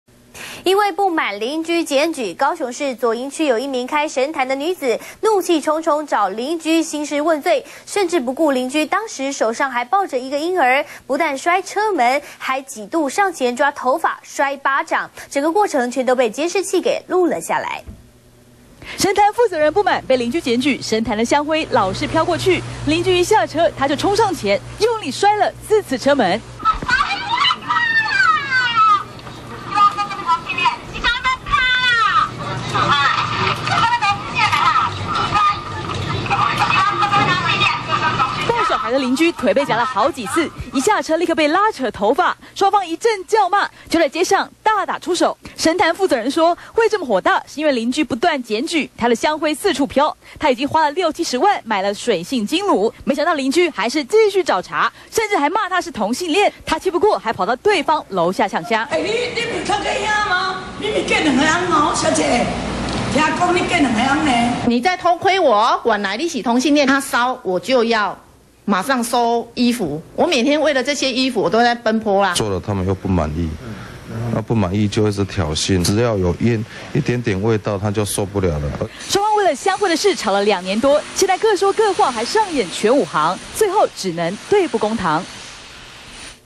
神壇香灰擾鄰 申訴反遭女壇主毆打
中視新聞》高雄市左營區一名開神壇的女子，因不滿鄰居檢舉，怒氣沖沖找鄰居興師問罪，甚至不顧鄰居當時手上抱著一個嬰兒，不但摔車門還幾度上前抓頭髮甩巴掌，整個過程全被監視器錄了下來。